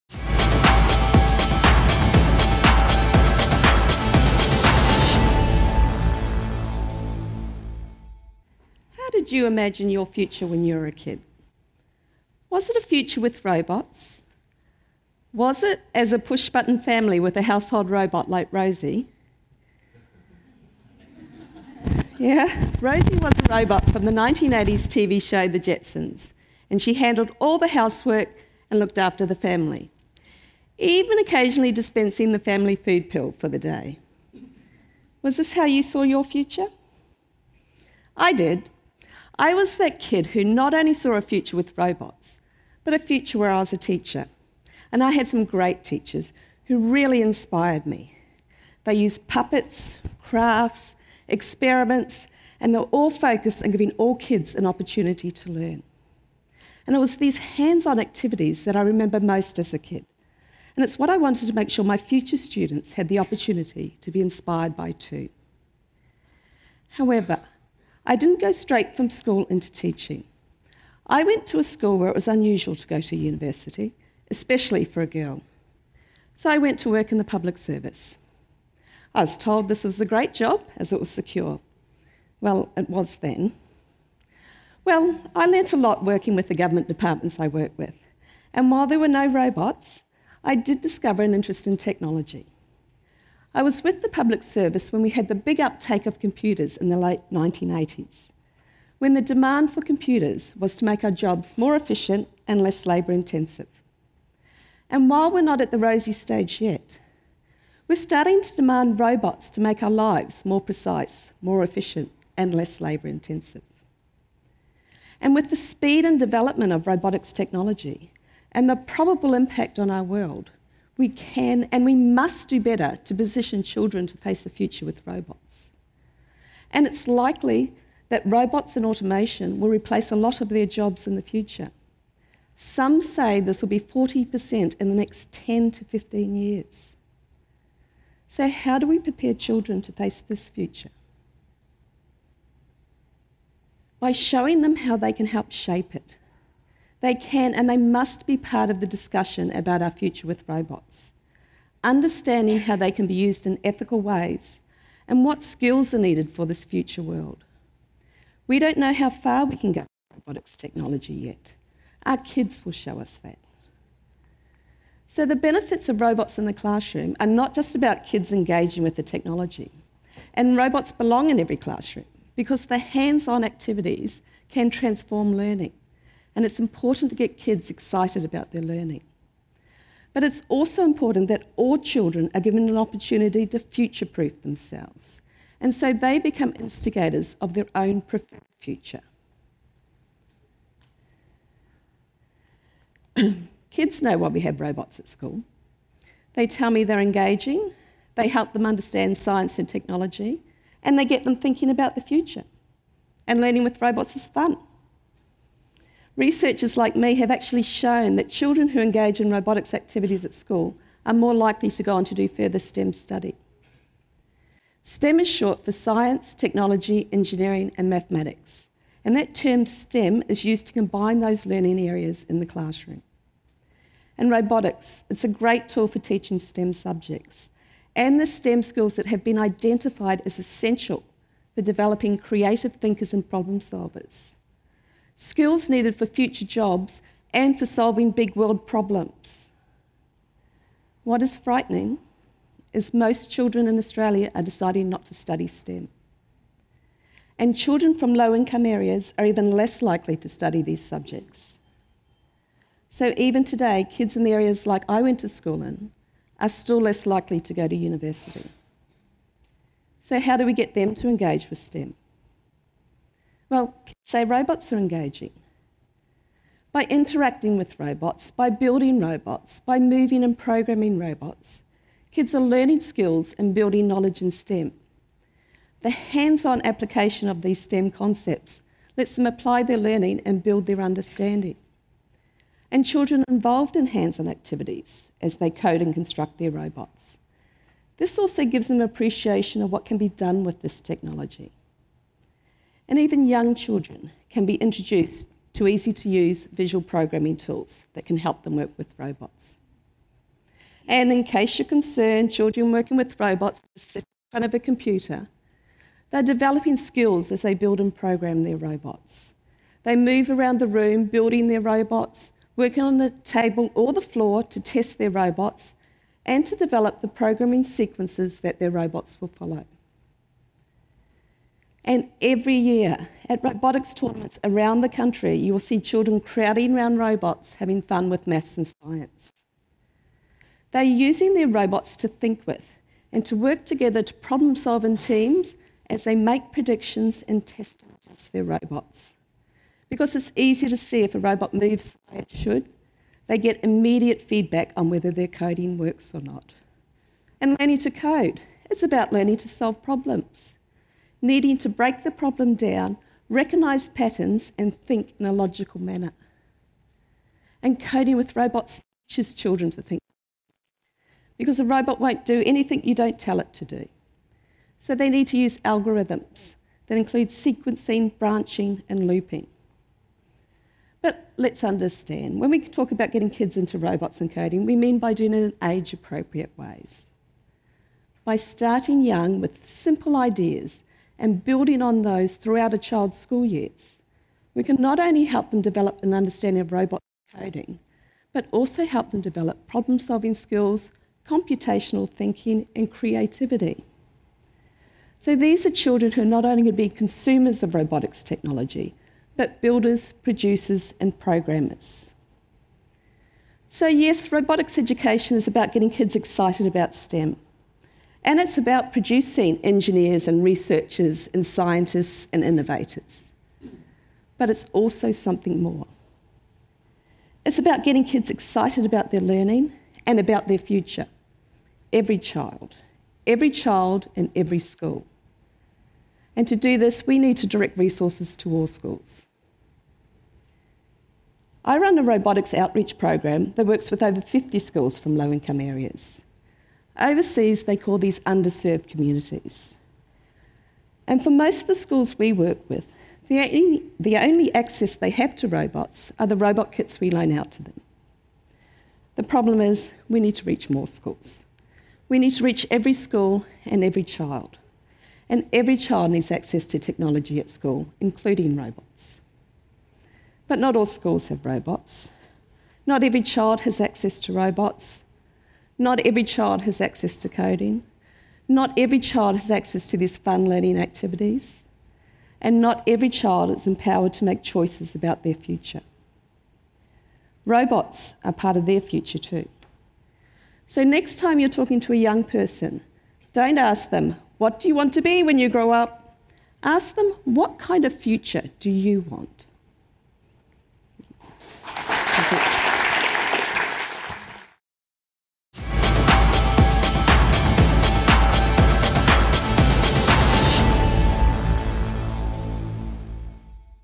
Robots need to go to school | Christina Chalmers | TEDxQUT
Kids get it, robots are fun and they are the future! So why aren't we doing more to ensure every student has the same opportunities to access them today?

Dr Christina Chalmers is a Senior Lecturer in Mathematics and Technologies Education in the School of Teaching Education and Leadership at Queensland University of Technology (QUT). Chris currently leads the Robotics@QUT project which is a support network developed to assist teachers and students from over 40 schools in low SES areas, engaging in robotics-based Science, Technology, Engineering and Mathematics (STEM) activities. Chris is involved in research projects focusing on technology, robotics, social skills, computational thinking, and STEM